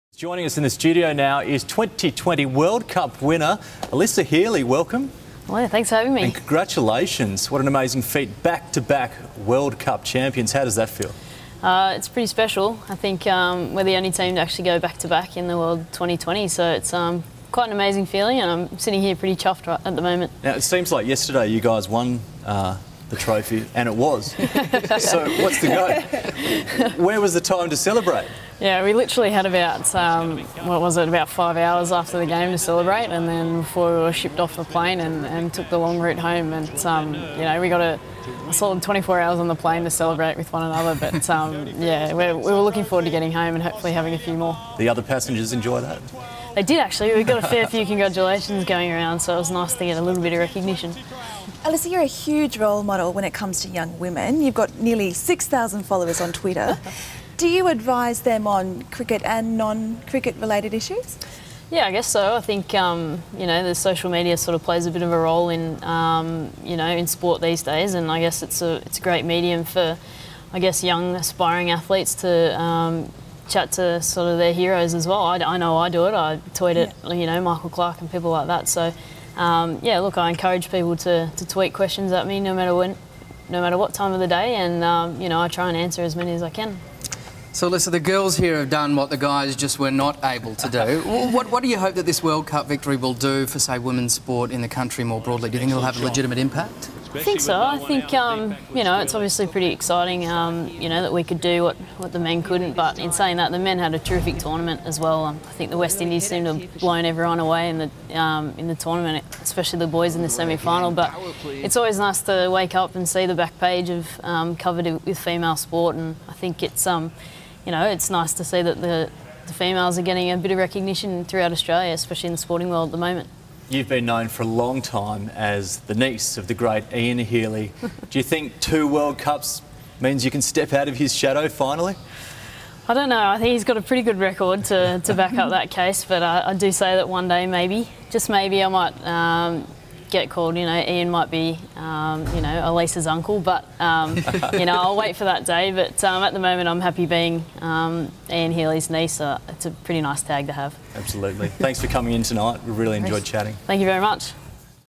Alyssa's world
Women's Twenty20 World Cup victory with Australian batter Alyssa Healy